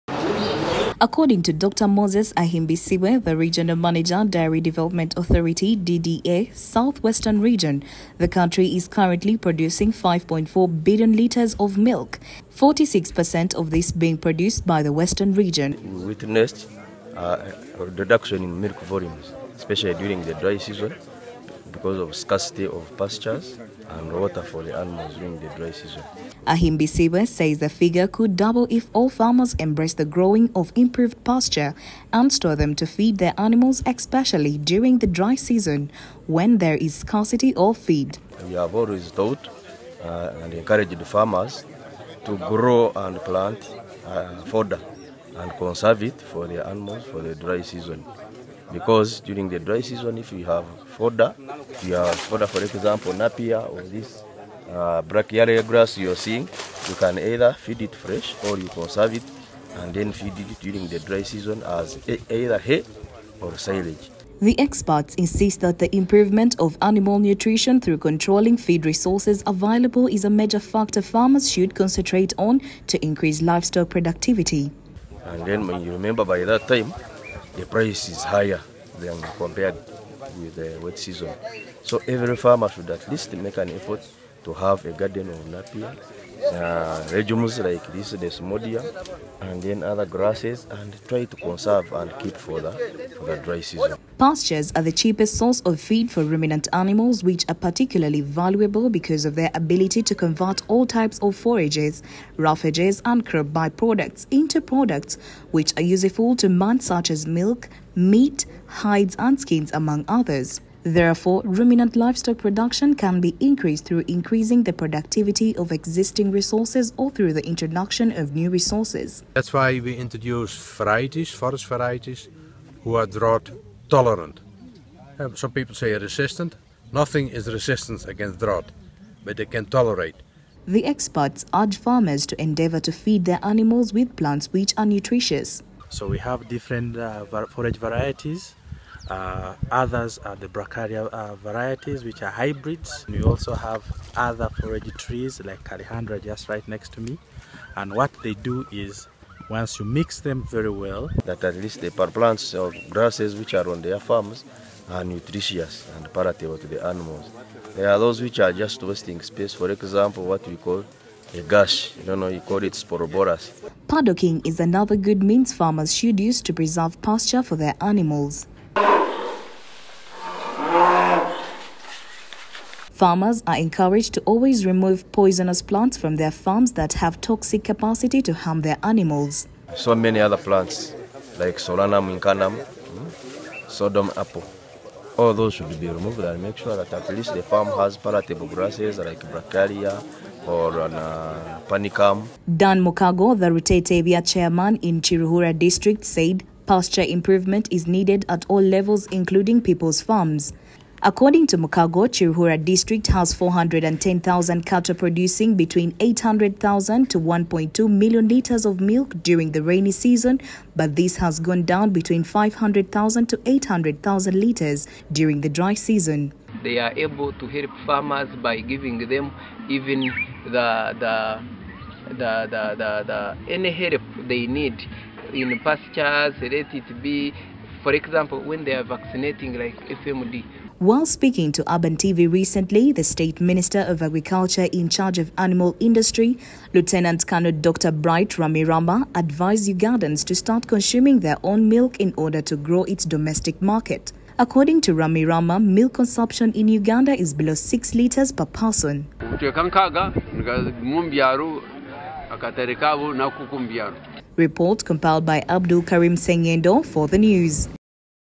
Farmers urged to invest in pasture growing
As the world continues to celebrate Dairy Month (June), farmers from Ankole region and other cattle corridor districts have been urged to invest in pasture growing and store feeds for their animals
With this move, farmers will not be affected by the dry season according to the agriculture experts.